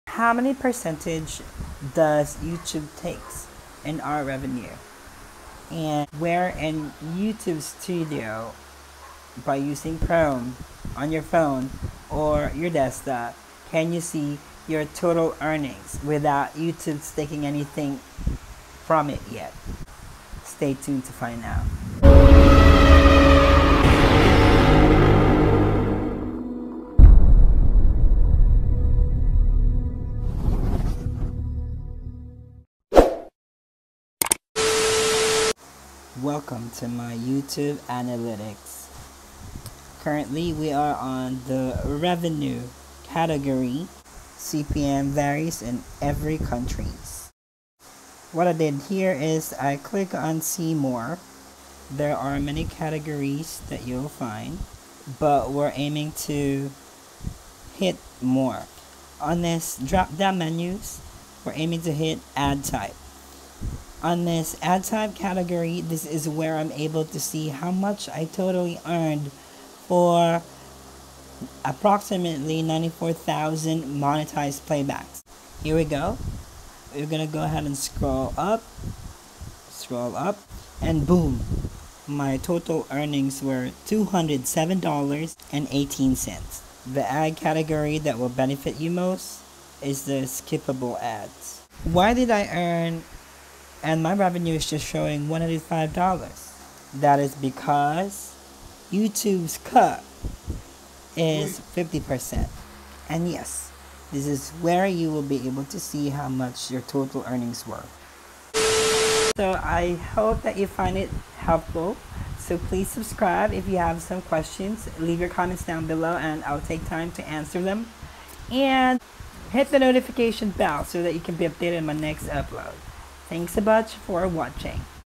How Much Does Youtube Earn From Your Revenue? | How To Read Your Youtube Analytics
Hi Ka-GAP! This is a short vlog about my Youtube Earnings and how much does youtube takes from it and where to find it in Youtube Studio using your Phone Browser or Desktop. Leave your questions down below. Thanks a bunch for watching!

Questions: YOUTUBE REVENUE

1. How much does youtube earn from our revenue?
2. Where to find youtube’s cut in Youtube Studio?
3. HOW TO READ YOUR YOUTUBE ANALYTICS

Device Used:

Iphone6s+